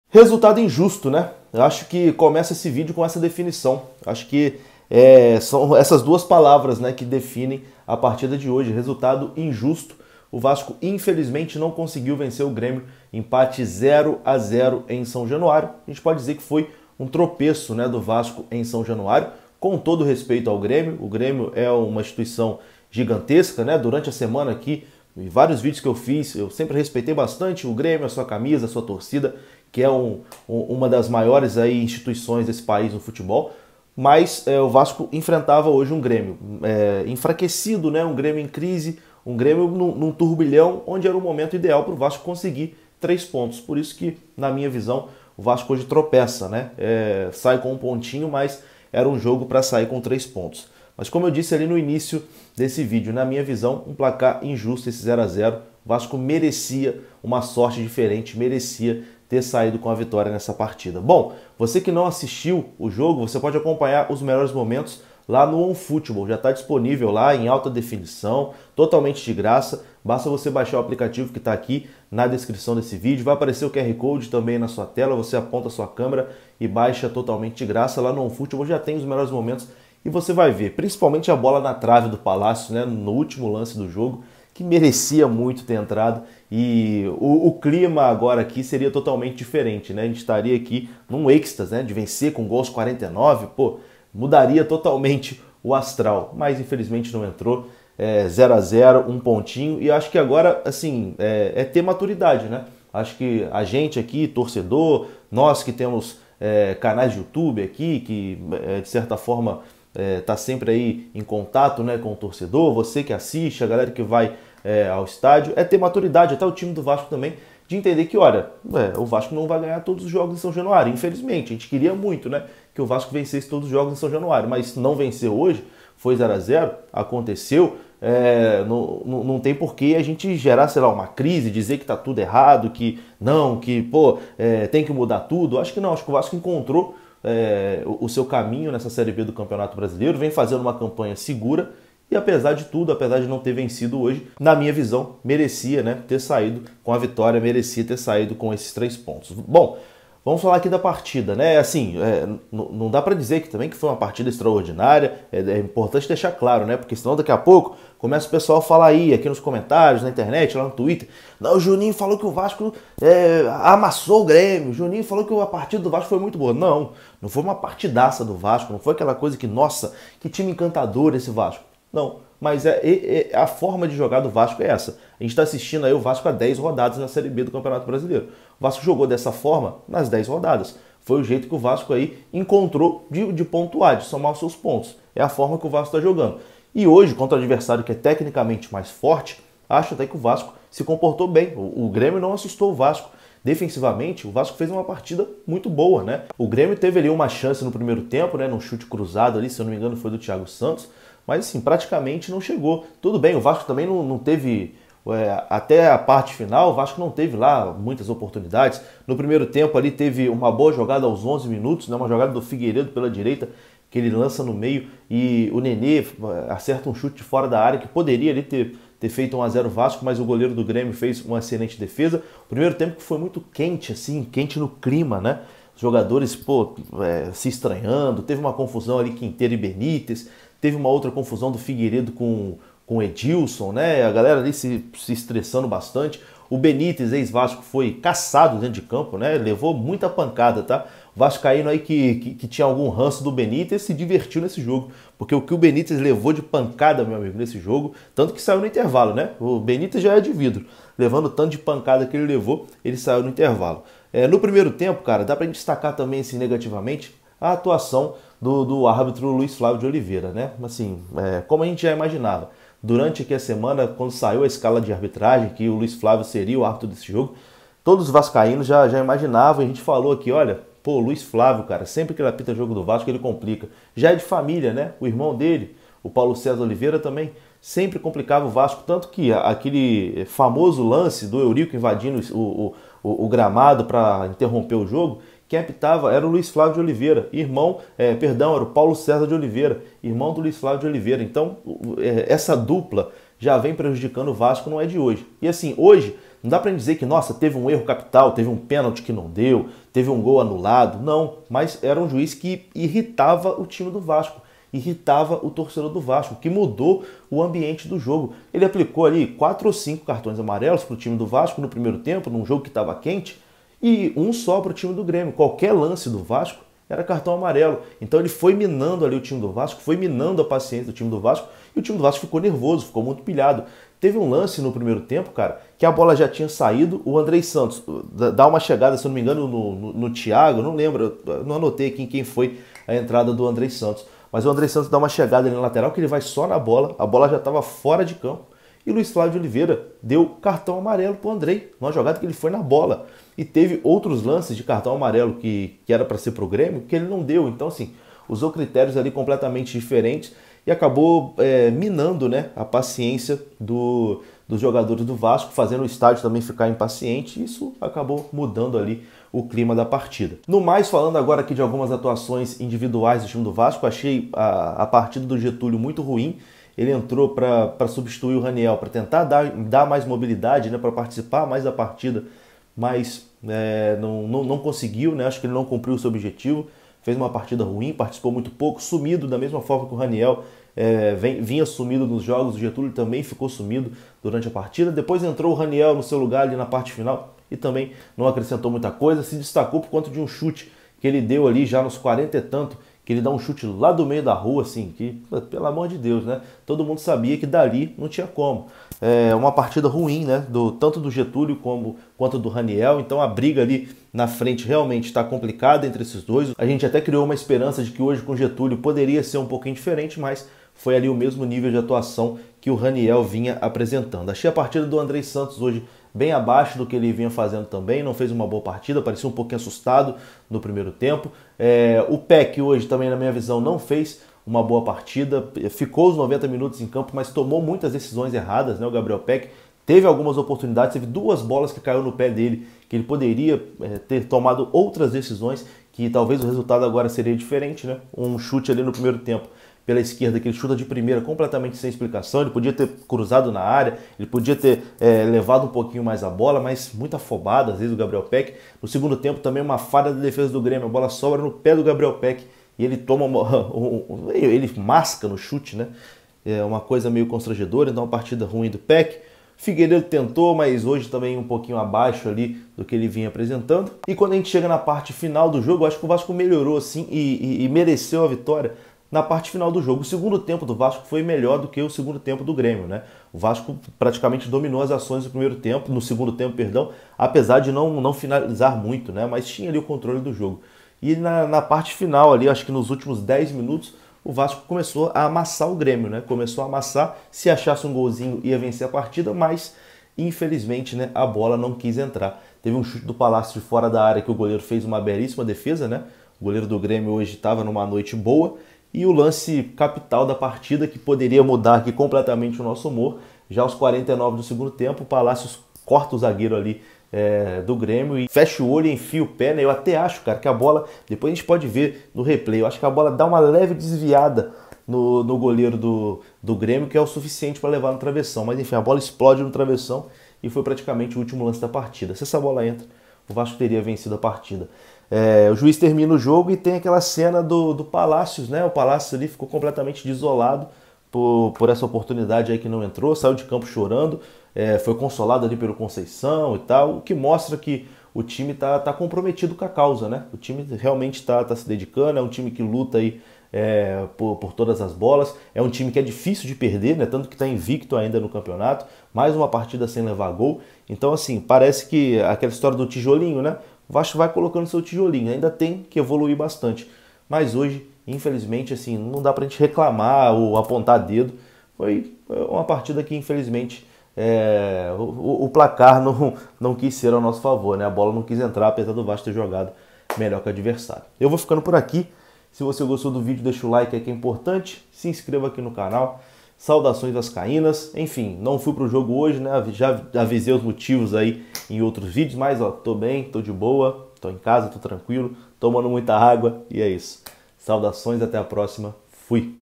VASCO 0 x 0 GRÊMIO | PLACAR INJUSTO DEMAIS! O VASCO MERECIA A VITÓRIA! SEGUIMOS JUNTOS!
Giro de Notícias com as últimas informações do Vasco. Confira!